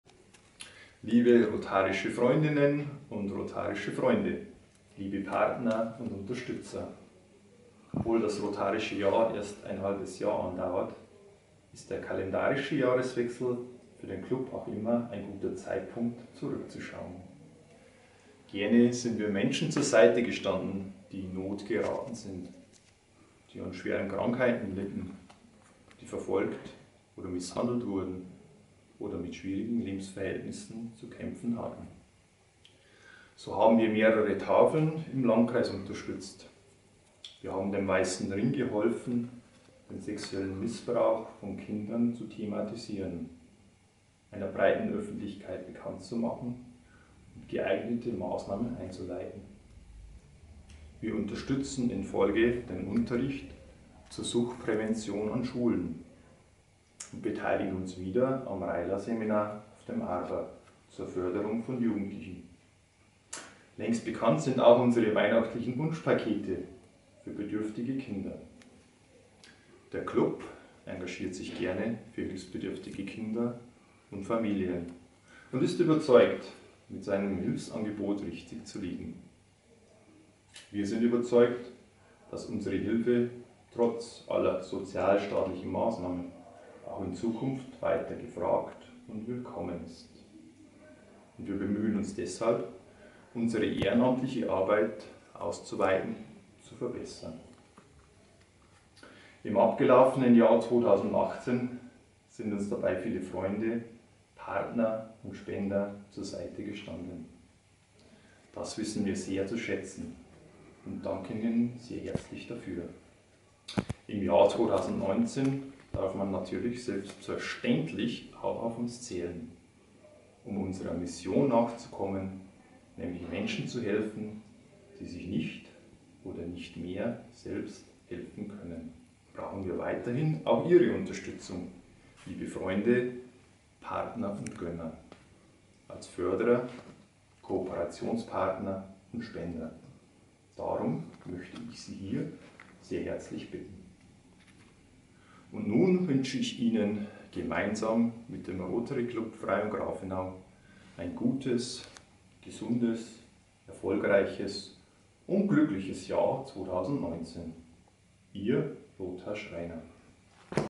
Jahreswechsel 2019
Grußbotschaft des Präsidenten des Rotary Clubs Freyung-Grafenau, Lothar Schreiner zum Jahr 2019